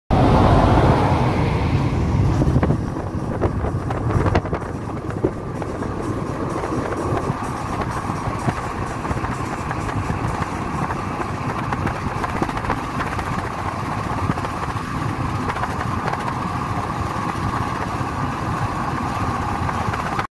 Cómo es el camino de LA PAZ a LOS CABOS. Baja California Sur 🇲🇽
Es un bellísima lugar.

Las carreteras muy bien cuidadas y los paisajes del mar.

Es hermoso 😍